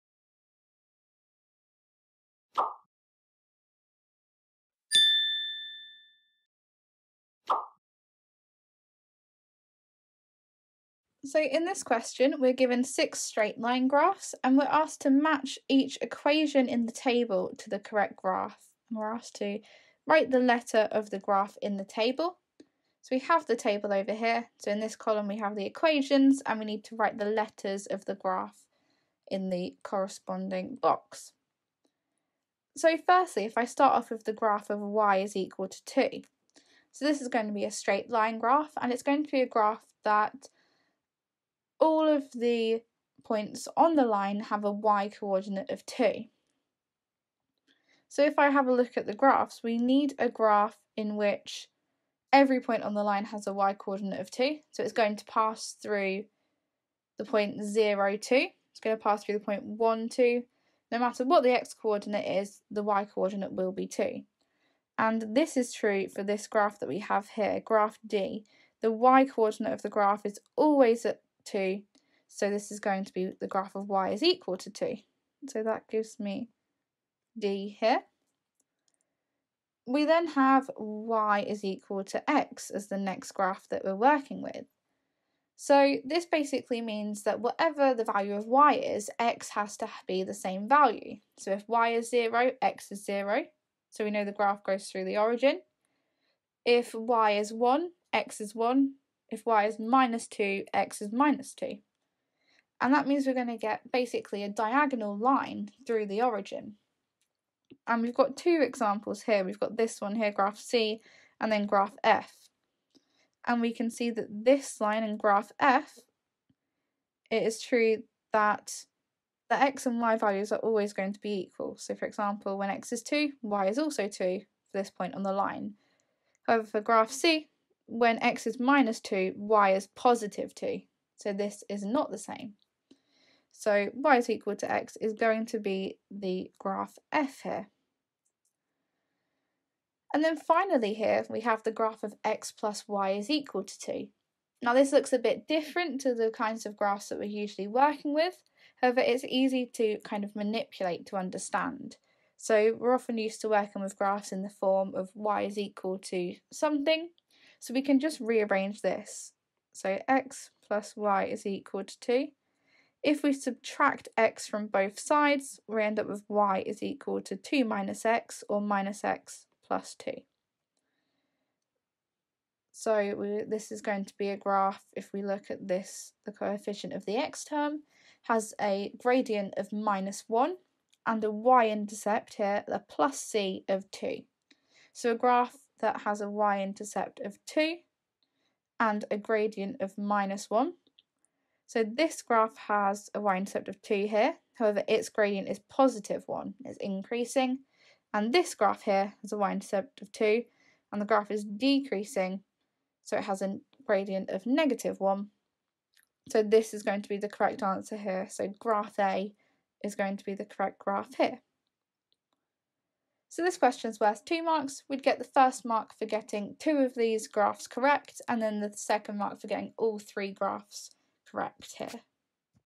Graphs of Linear Equations (Foundation) - Q5 | Maths GCSE | PMT Education
This is the video solution for our Mathematics GCSE Graphs of Linear Equations (Foundation) Questions by Topic. You can watch all the videos for this question set in this playlist

You can also download a PDF copy of the Questions by Topic, the mark scheme and model answers